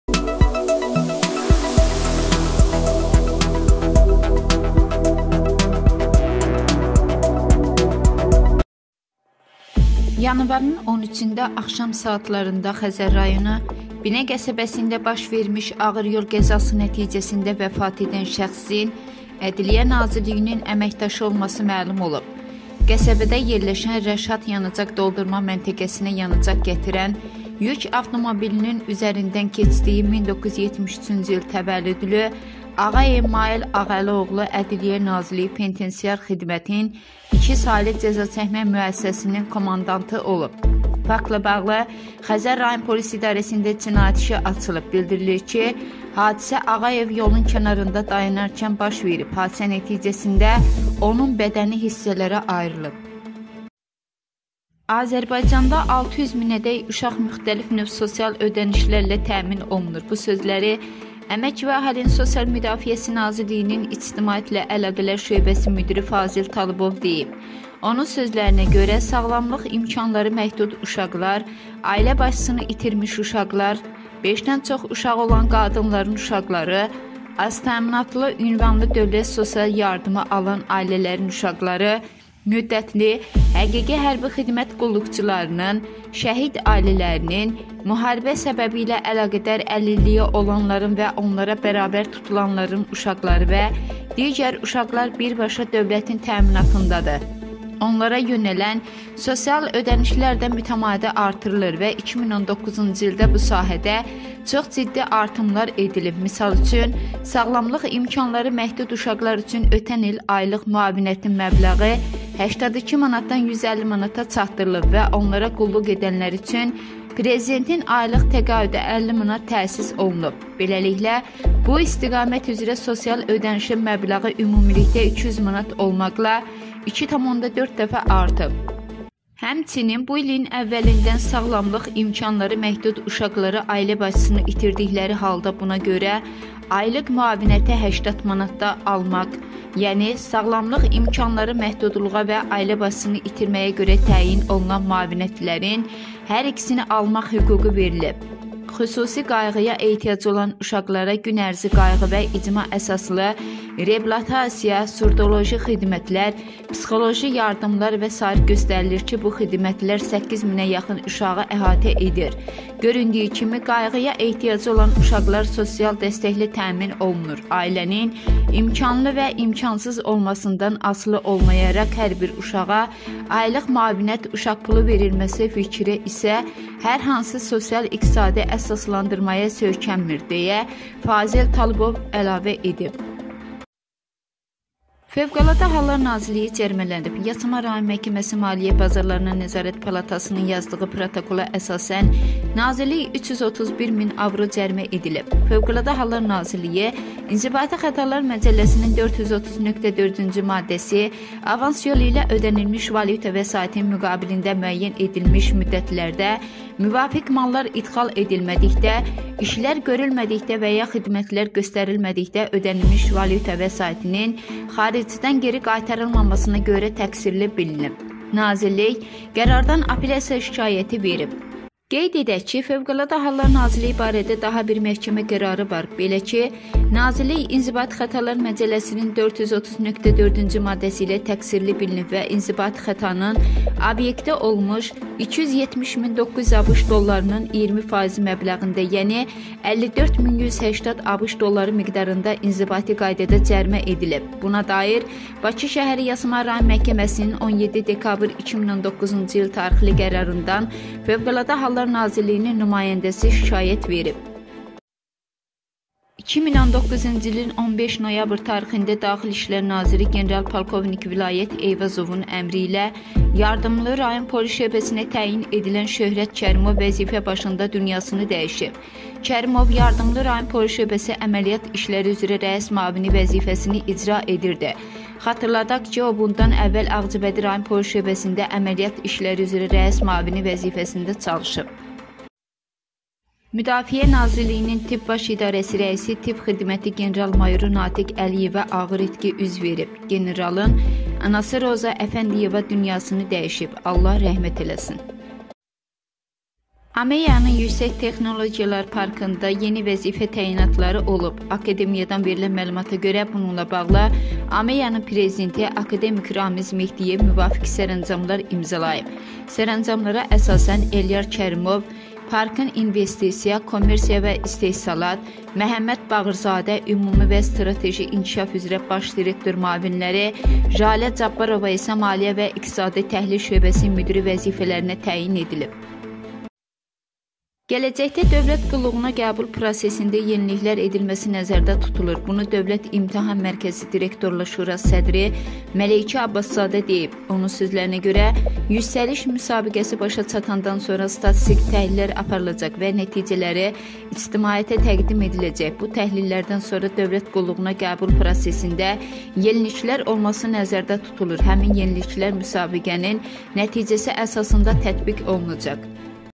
Təcili Bu Uşaqlara Pul Ayrılır Kamələddin Heydərova Ağır Zərbə
Əmək və Əhalinin Sosial Müdafiəsi Nazirliyinin İctimaiyyətlə əlaqələr şöbəsinin müdiri Fazil Talıbov deyib.

Onun sözlərinə görə, sağlamlıq imkanları məhdud uşaqlar, ailə başçısını itirmiş uşaqlar, beşdən çox uşağı olan qadınların uşaqları, aztəminatlı (ünvanlı dövlət sosial yardımı alan) ailələrin uşaqları, müddətli həqiqi hərbi xidmət qulluqçularının, şəhid ailələrinin, müharibə səbəbi ilə əlaqədar əlilliyi olanların və onlara bərabər tutulanların uşaqları və digər uşaqlar birbaşa dövlətin təminatındadır.

"Onlara yönələn sosial ödənişlər də mütəmadi artırılır və 2019-cu ildə bu sahədə də çox ciddi artımlar edilib. Misal üçün, sağlamlıq imkanları məhdud uşaqlar üçün ötən il aylıq müavinətin məbləi 82 manatdan 150 manata çatdırılıb və onlara quluq edənlər üçün Prezidentin aylıq təqaüdü (50 manat) təsis olunub. Beləliklə, bu istiqamət üzrə sosial ödənişin məbləği ümumilikdə 200 manat olmaqla, 2,4 dəfə artıb. Həmçinin bu ilin əvvəlindən sağlamlıq imkanları məhdud uşaqlara ailə başçısını itirdikləri halda buna görə aylıq müavinəti (80 manat) də almaq, yəni sağlamlıq imkanları məhdudluğa və ailə başçısını itirməyə görə təyin olunan müavinətlərin hər ikisini almaq hüququ verilib.

Xüsusi qayğıya ehtiyacı olan uşaqlara günərzi qayğı və icma əsaslı reabilitasiya, surdoloji xidmətlər, psixoloji yardımlar və s. göstərilir ki, bu xidmətlər 8 minə yaxın uşağı əhatə edir. Göründüyü kimi, qayğıya ehtiyacı olan uşaqlar sosial dəstəklə təmin olunur, ailənin imkanlı və imkansız olmasından asılı olmayaraq hər bir uşağa aylıq müavinət (uşaq pulu) verilməsi fikri isə hər hansı sosial-iqtisadi əsaslandırmaya söykənmir",-deyə Fazil Talıbov əlavə edib.

 Yasamal rayon Məhkəməsi Maliyyə Bazarlarına Nəzarət Palatasının yazdığı protokola əsasən, nazirlik 331 000 avro cərimə edilib.

FHN İnzibati Xətalar Məcəlləsinin 430.4-cü maddəsi (avans yolu ilə ödənilmiş valyuta vəsaitinin müqabilində müəyyən edilmiş müddətlərdə müvafiq mallar idxal edilmədikdə, işlər görülmədikdə və ya xidmətlər göstərilmədikdə ödənilmiş valyuta vəsaitinin xaricdən geri qaytarılmamasına görə) ilə təqsirli bilinib.

Nazirlik qərardan apelyasiya şikayəti verib.

“Qafqazinfo” əlavə edib ki, Fövqəladə Hallar Nazirliyi barədə daha bir məhkəmə qərarı var. Belə ki, nazirlik İnzibati Xətalar Məcəlləsinin 430.4-cü maddəsi ilə təqsirli bilinib və inzibati xətanın obyekti olmuş 270900,00 ABŞ dollarının iyirmi faizi məbləğində, yəni 54180 (əlli dörd min yüz səksən) ABŞ dolları miqdarında inzibati qaydada cərimə edilib.

Buna dair Bakı şəhəri Yasamal Rayon Məhkəməsinin 17 dekabr 2019-cu il tarixli qərarından Fövqəladə Hallar Nazirliyinin nümayəndəsi şikayət verib.